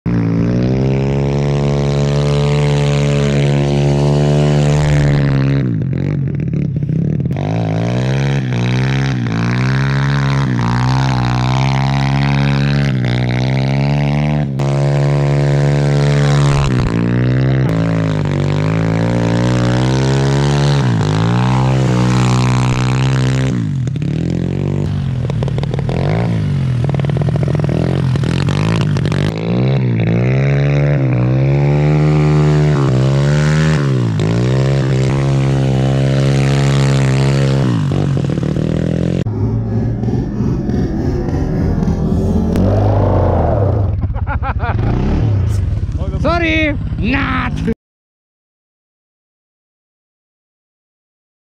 winter games
cleaning the snow with a drift trike